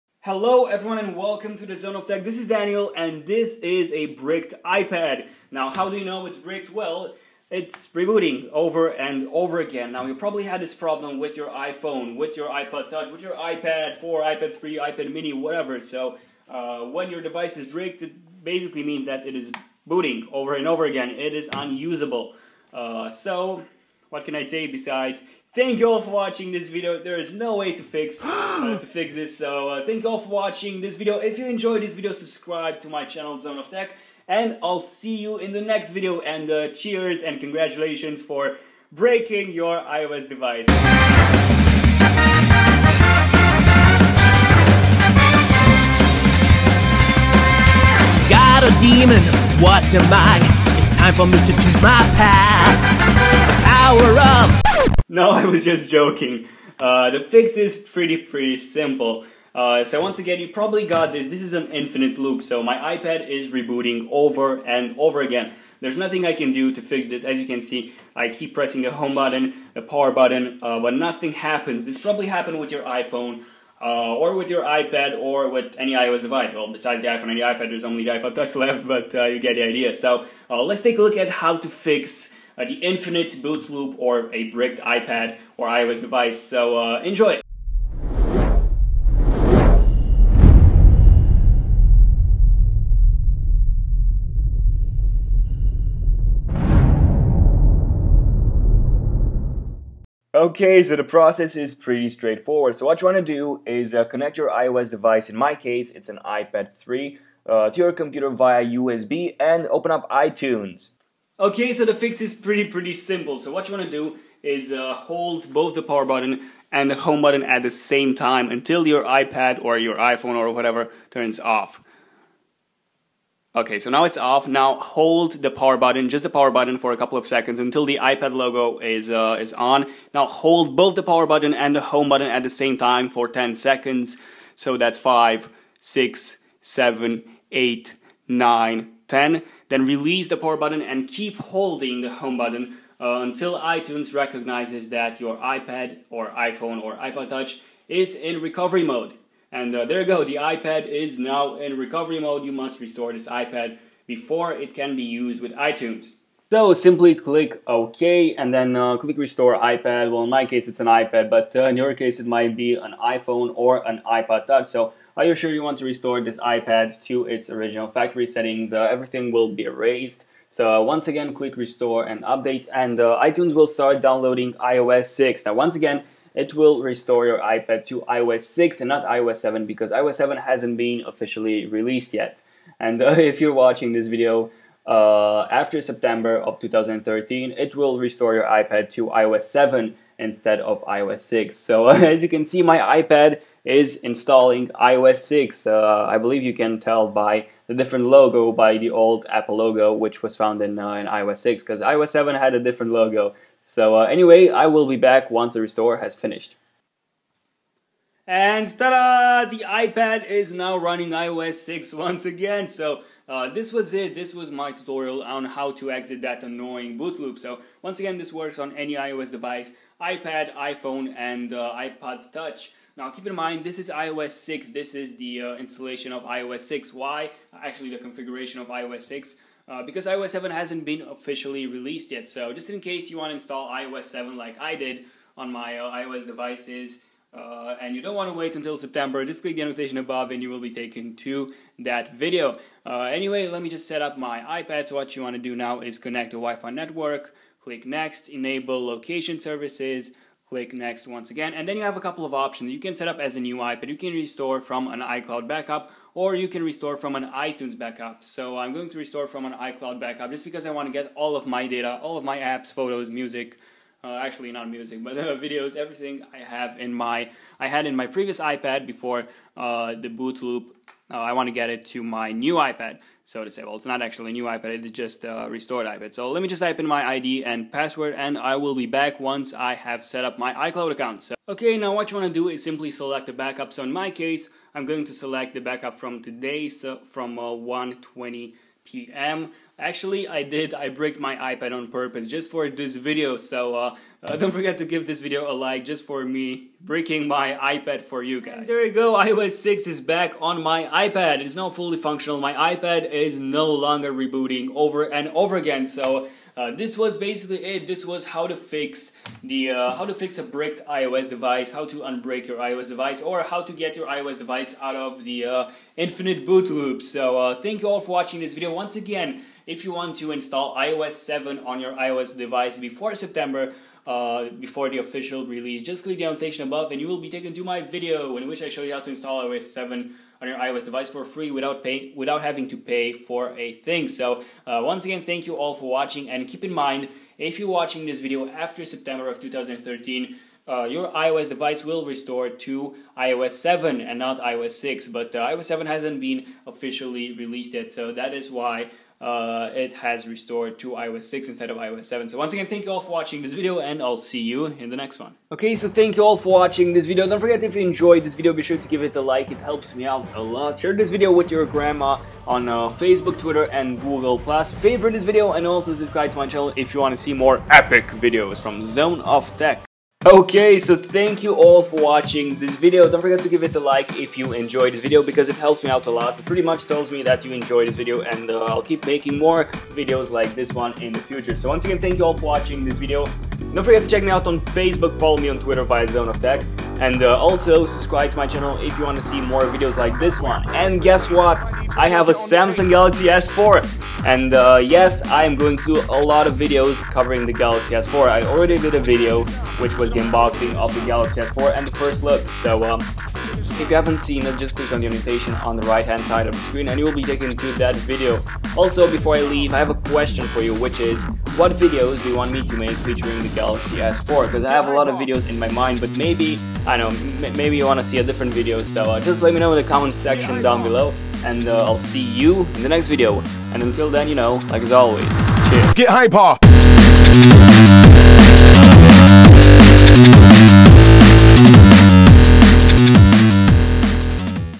HOW TO: Unbrick iPhone | EXIT Boot Loop on the iPhone, iPad and iPod Touch | FIX infinite reboot
Unbrick your iPhone | FIX INFINITE reboot (exit infinite boot loop) on the iPhone , iPad and iPod Touch

*Can we get this video to 100 likes?* :D

INSTALL iOS 7 BETA 2 & BETA 3 on the iPhone 4/4s/5 and iPod Touch

BE A BUDDY TODAY!